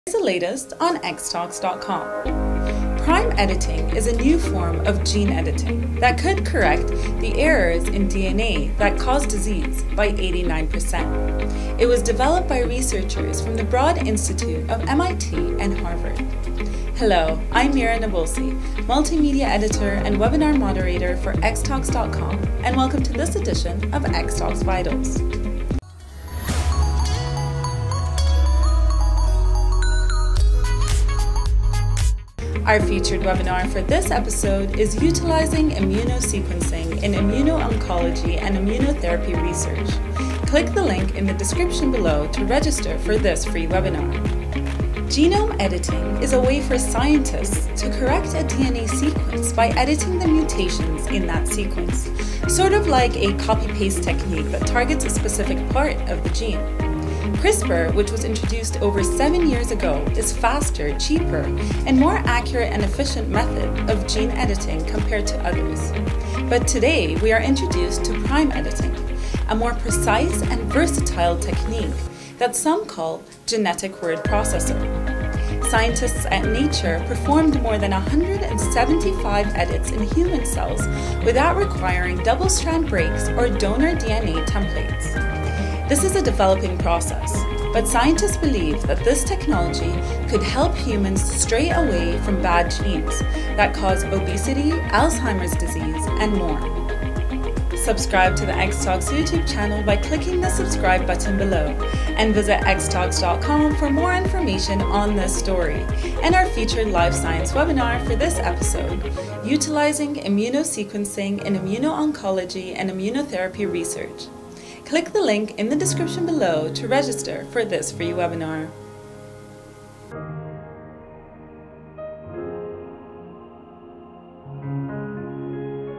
Prime Editing Could Correct 89 Percent of Disease Causing Mutations
A technology called prime editing has been introduced to accurately re-code DNA sequences. This is a variant of CRISPR, which is a technique used by scientists to re-edit a genetic code in order to alter a mutation and eliminate errors in the sequence, such as mutations causing sickle cell anemia.

Listen live and interact with featured speakers on our upcoming Life Science webinars